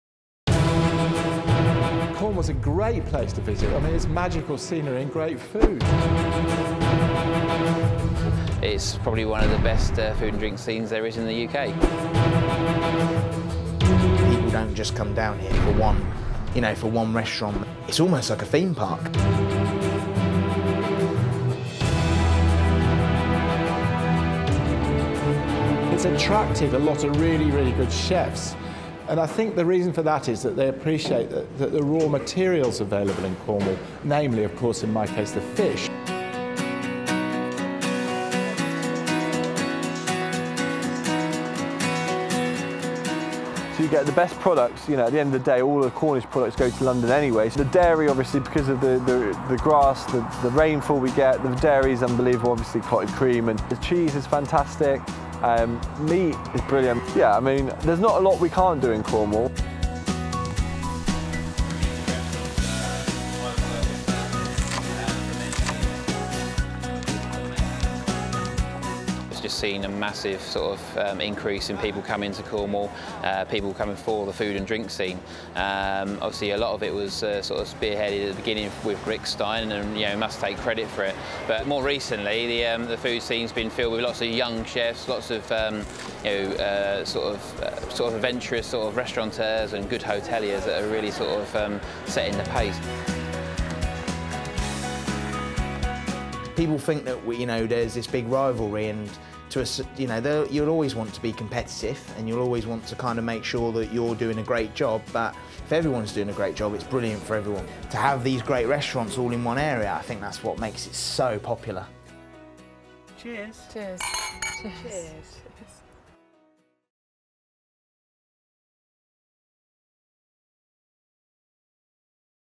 Cornwall's chefs on Cornwall's food scene
Cornwall is now known as a prime foodie destination.

In this fab short film, Rick Stein and son Jack, Nathan Outlaw and Paul Ainsworth are among some of the region's top chefs championing the county’s abundant seafood and fresh produce. They tell us why the Cornish Food scene has developed to rival anywhere else in the UK.

Publisher: Visit Cornwall
Producer: Soundview Media